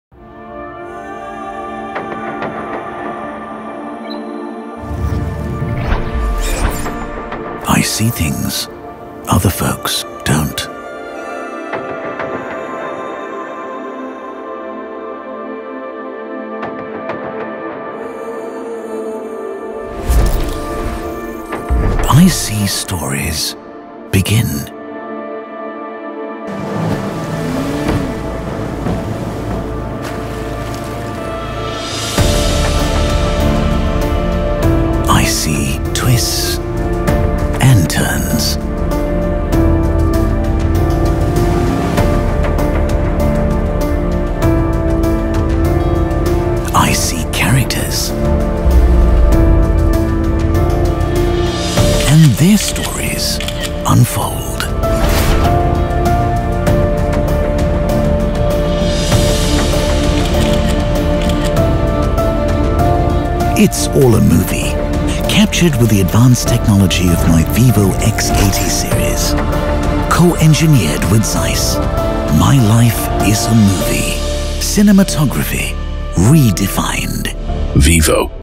vivoX80Series | CinematographyRedefined
Capture movie-like stills and crystal-clear videos that bring alive every moment with the vivo X80 series, co-engineered with ZEISS.